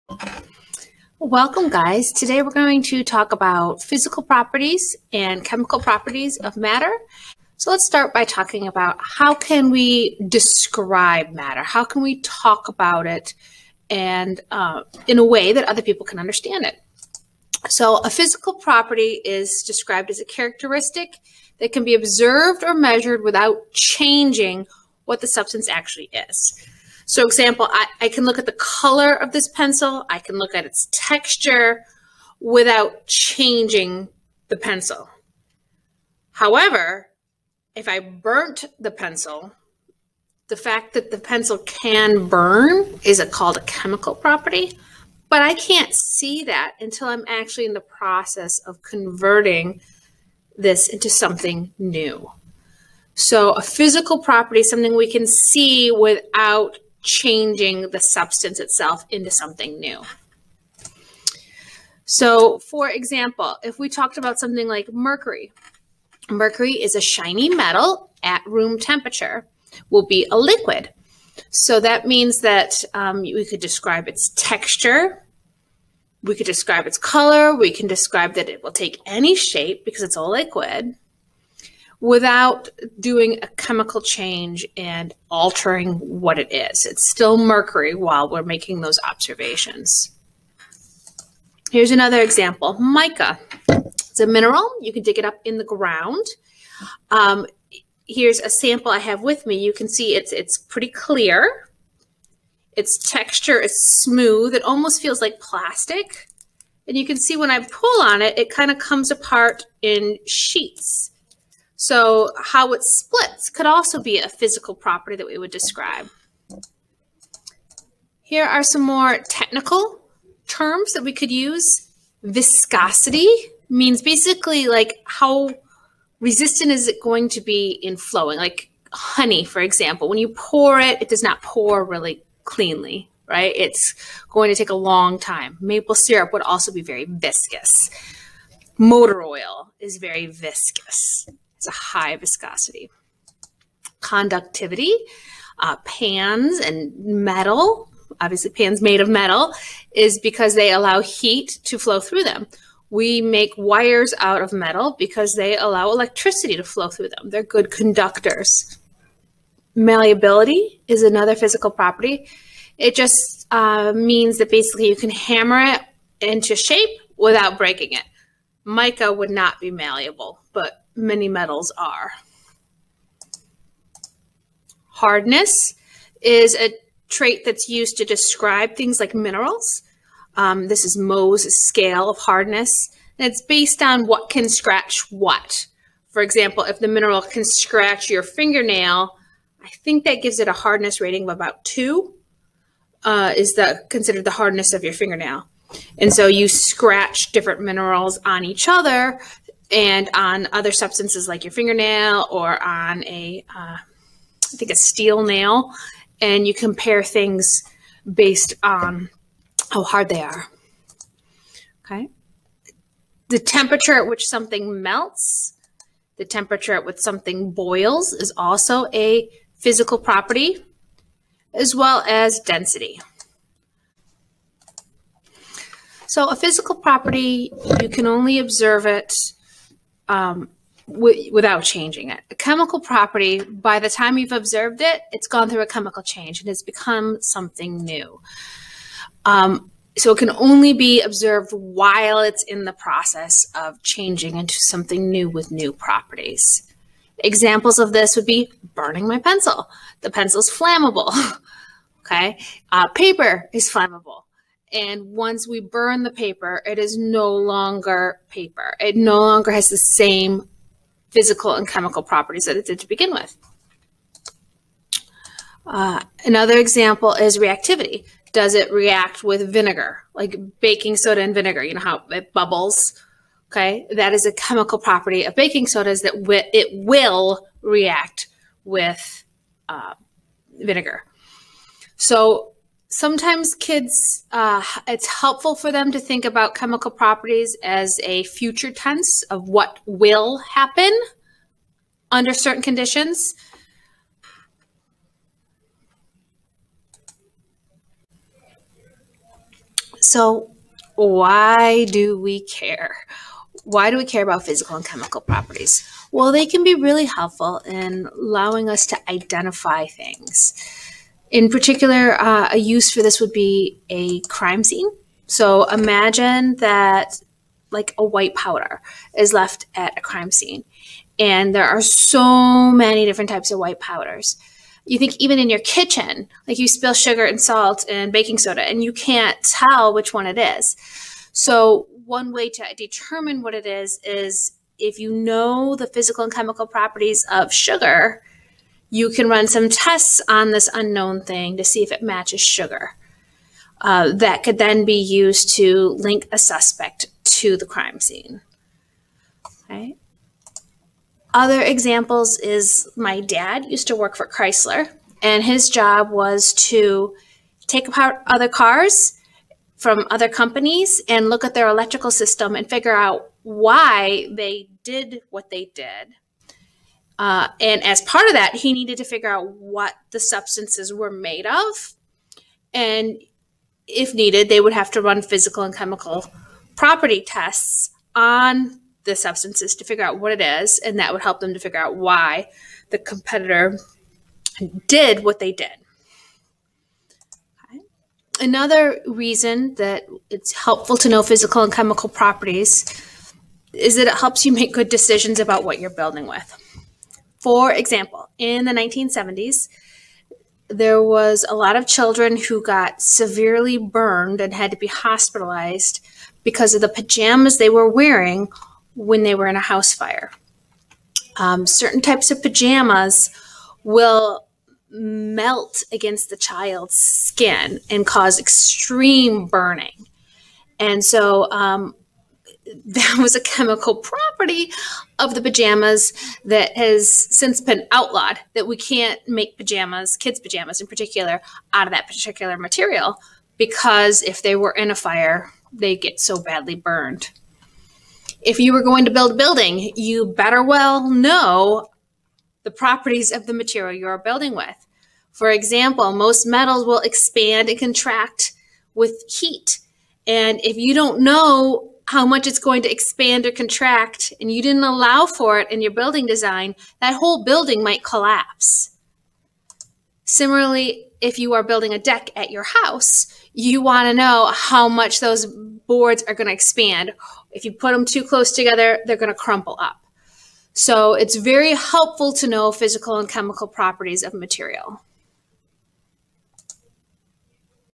Physical and Chemical Properties
The difference between physical and chemical properties is described as well as why it is important and how the information could be used.